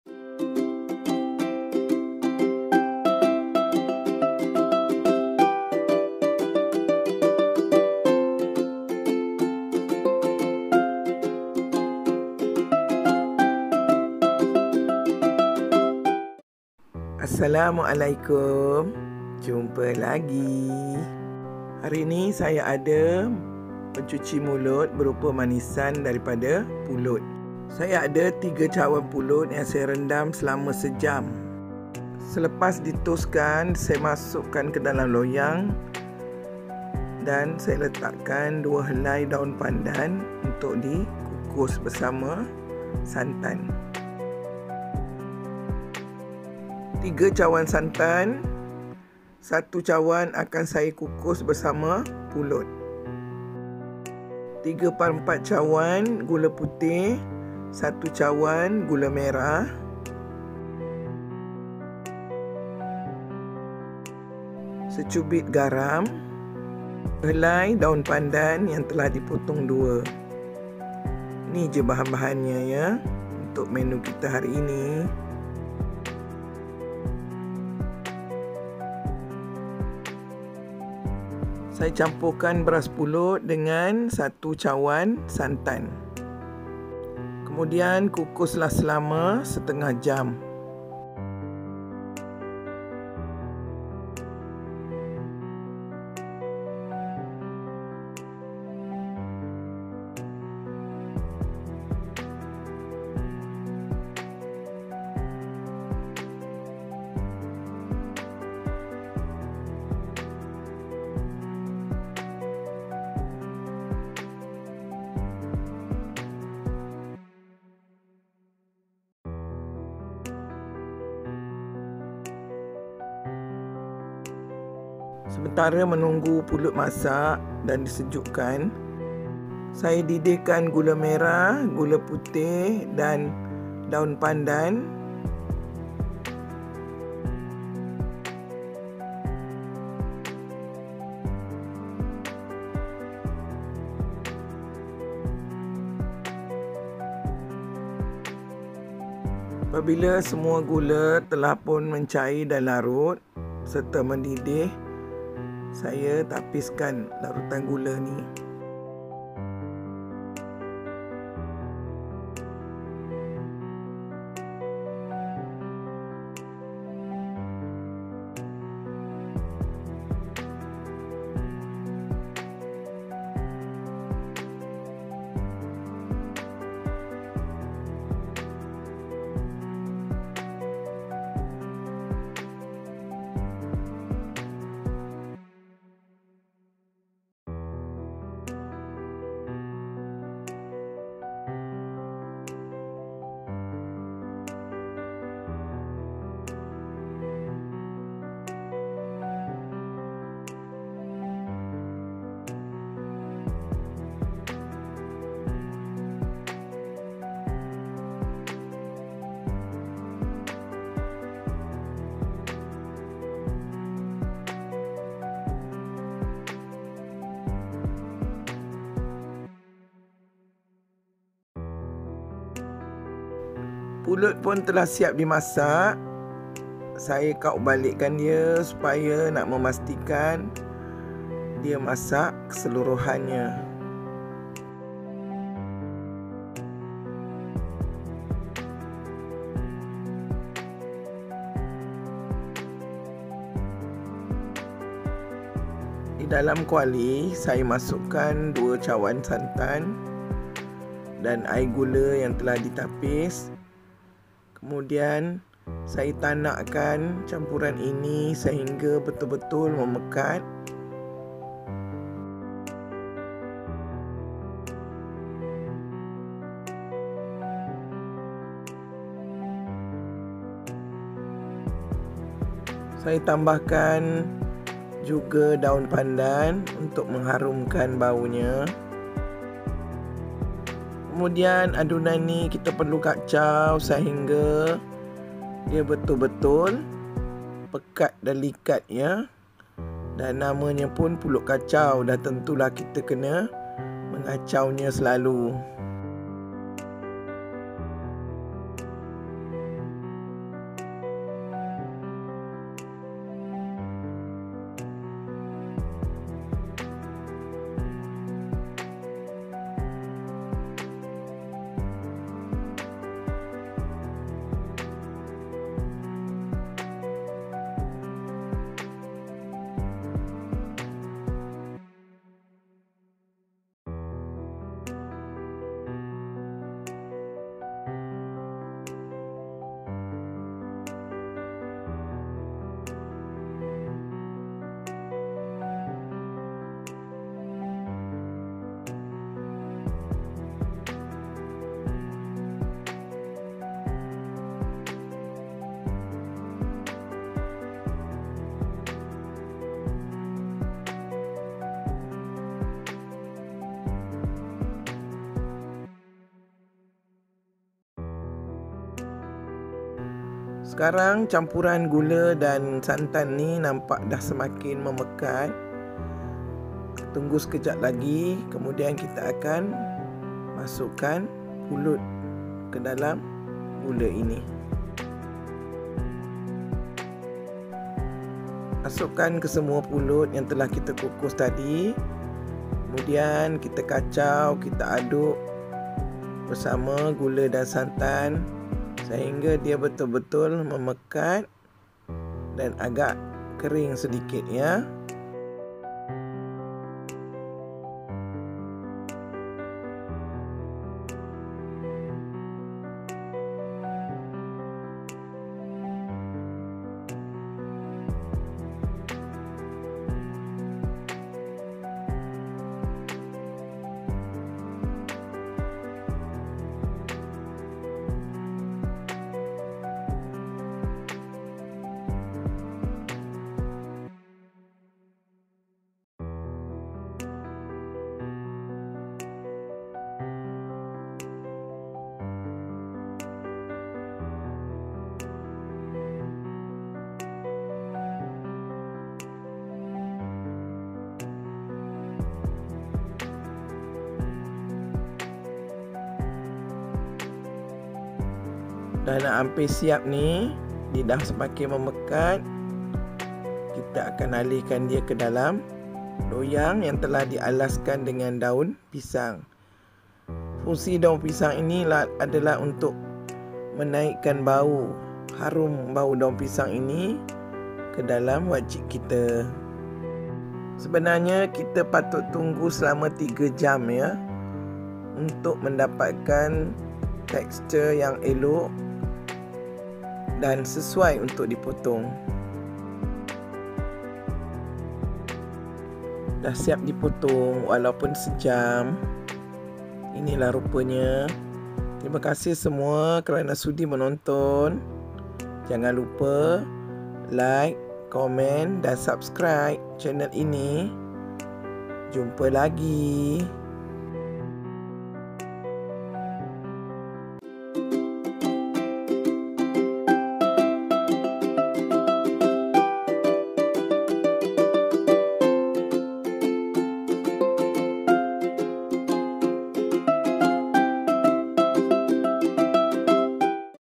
Wajik @ Pulut Kacau || lemak manis || haruman daun pandan, gula merah dan daun pisang memikat selera
Sungguh memikat rasa wajik ini, lemak dan penuh haruman alami.